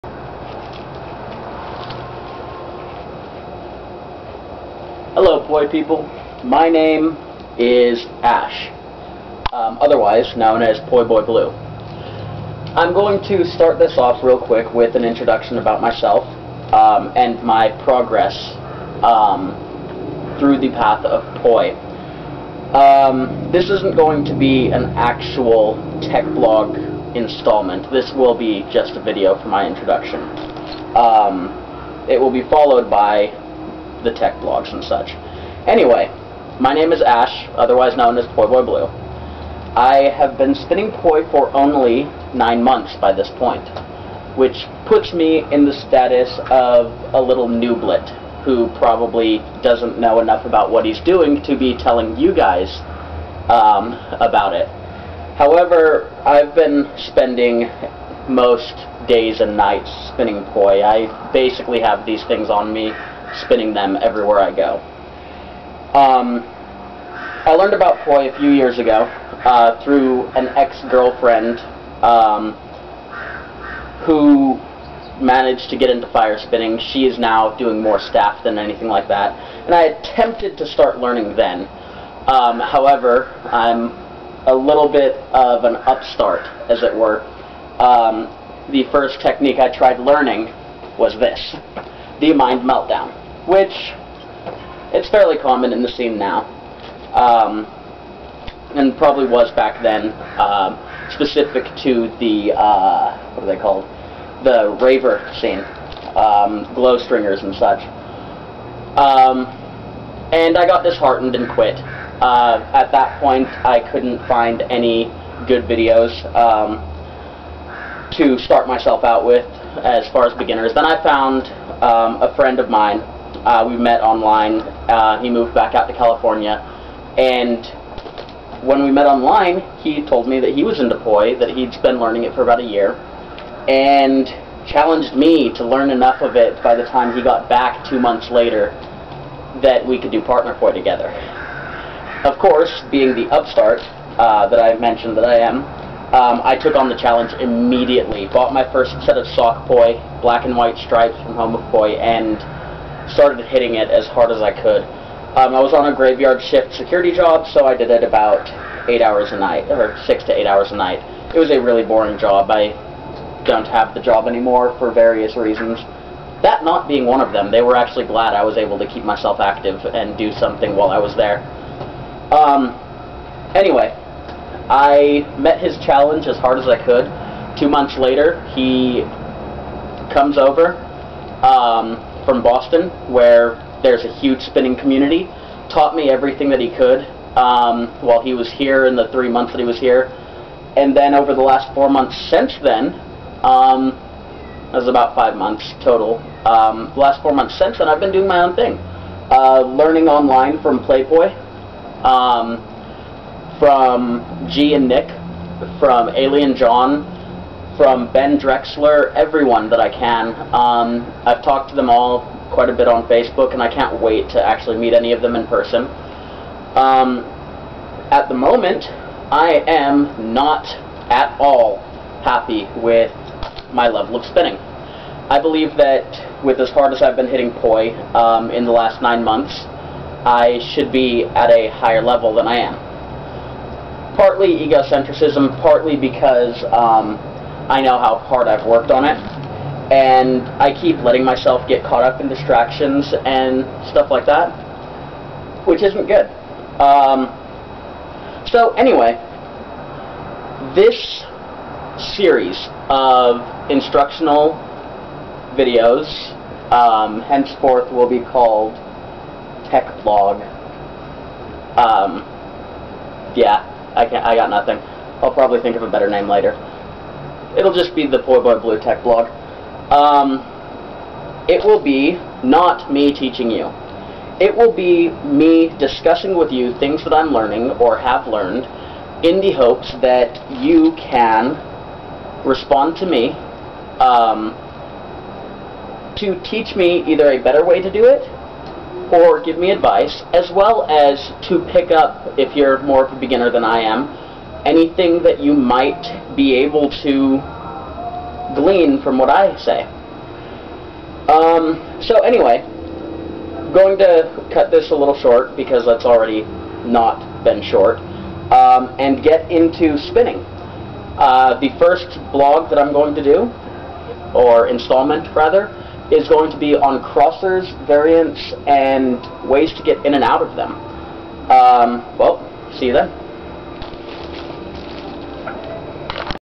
PoiBoyBlue TechniFlow Blog- Part 0: Introduction
Introducing myself and what I hope to achieve with the upcoming set of tutorials.